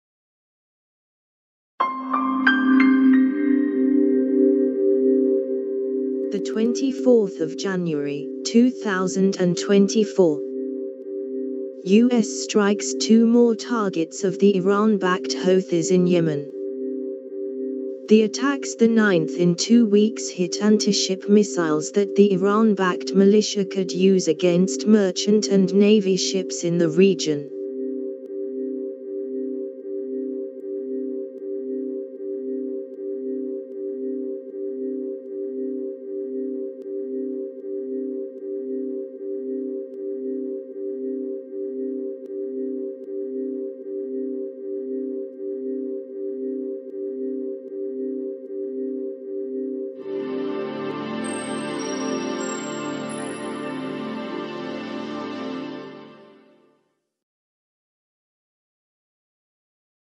[Latest]U.S. Strikes 2 More Targets of the Iran-Backed Houthis in Yemen
[Contents]
The attacks — the ninth in two weeks — hit anti-ship missiles that the Iran-backed militia could use against merchant and Navy ships in the region.